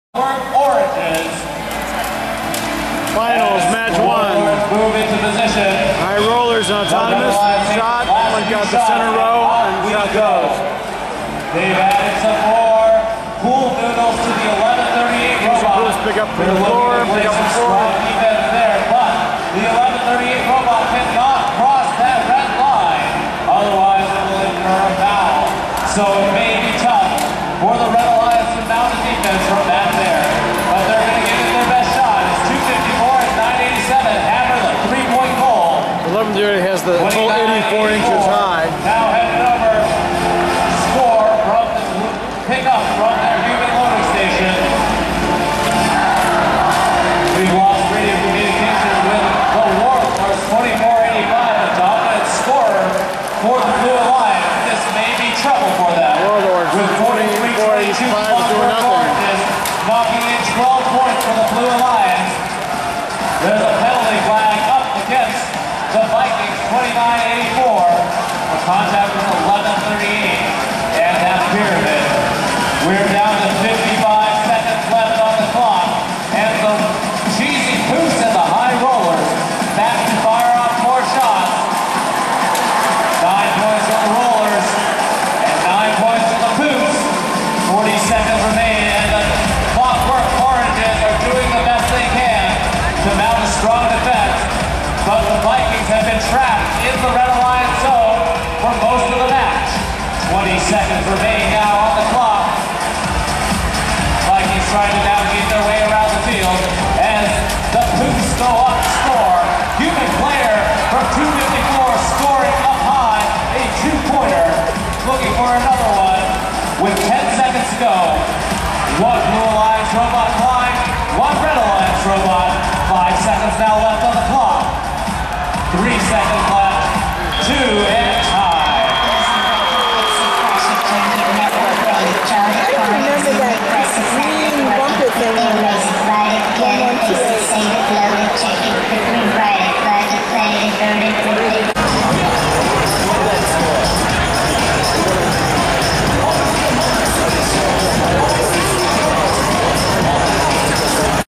FIRST 2013 San Diego Final-1
FIRST Robotics FRC 2013 Ultimate Ascent San Diego Regional, Match # Final-1. #1 RED (181 pt)(drivers on left) frc987 frc254 frc1138  vs. #3 BLUE (114 pt)(drivers on left) frc2984 frc2485 frc4322
Code Orange, Team 3476 was 4th pick, 2013 San Diego Semi-finalist and GM Industrial Design Award.